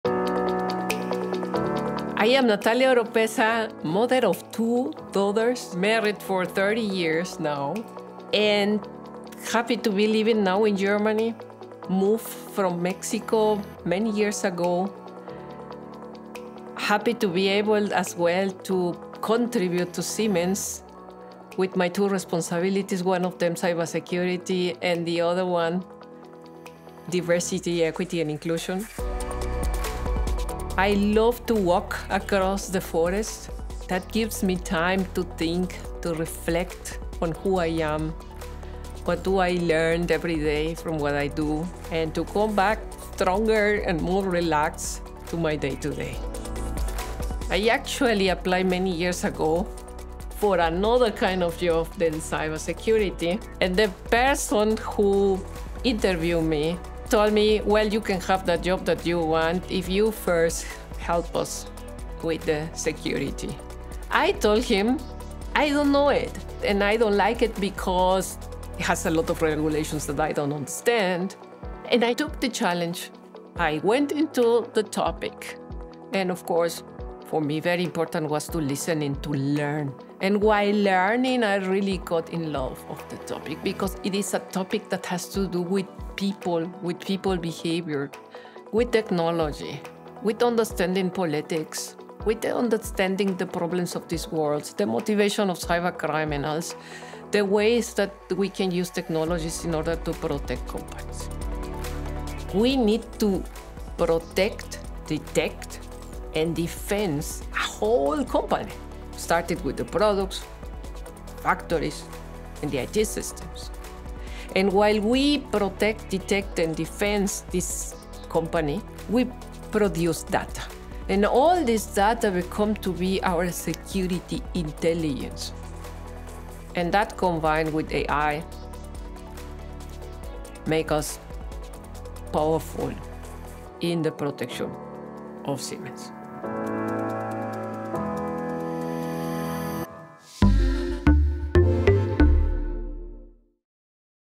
Female talents and leaders in cybersecurity at Siemens: Natalia Oropeza
In this video, Natalia Oropeza, Global Chief Cybersecurity Officer and Chief Diversity Officer at Siemens AG, shares inspiring insights about her work in SiemensCyber with TeamSiemens and why it’s worth taking on the challenge.